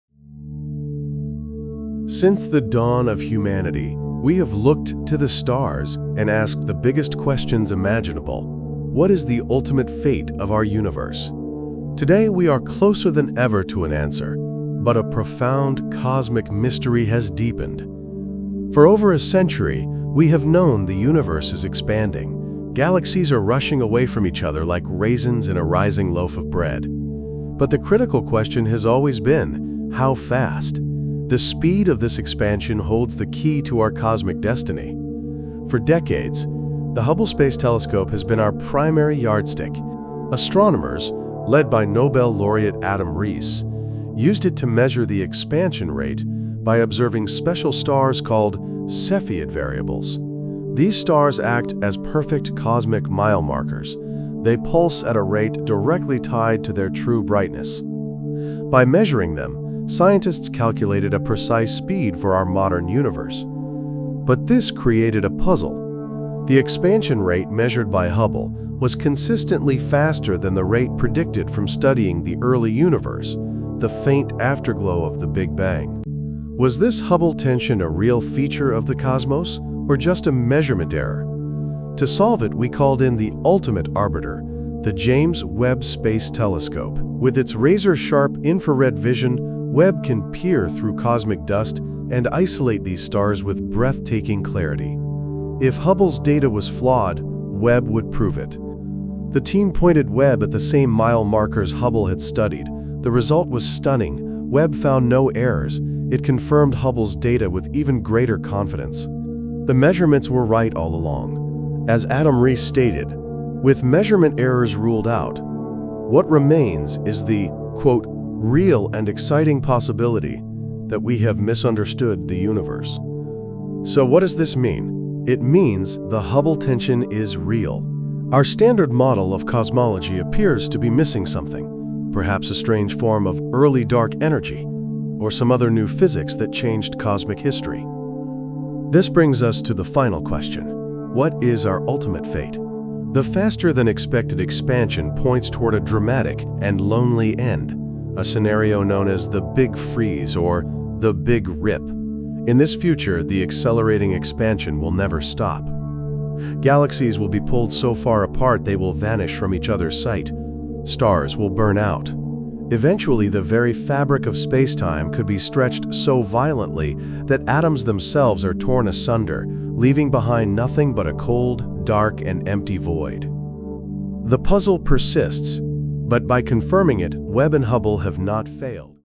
The Expanding Universe Problem and Quantum Parallel Universes
For years we've known that our universe is expanding, but the Hubble Tension has plagued scientists for almost as long: two differing ways of calculating this rate of expansion, both well understood, give us differing conclusions.  In this video, the James Webb Space Telescope weighs in, can it resolve this tension after so many years?  We also discuss the quantum weirdness of wavefunctions: does wave function collapse lead to parallel universes? Could we ever hope to prove such a theory?